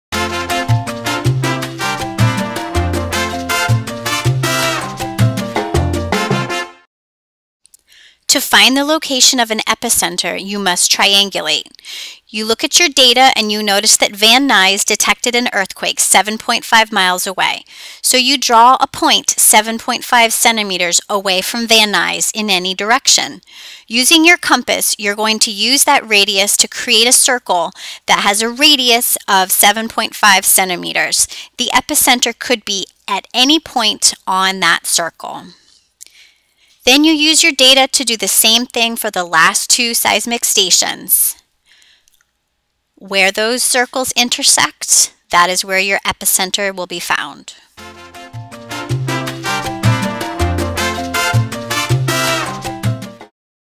Triangulation
Triangulation is a way of using data from three locations in order to locate the epicenter of an earthquake.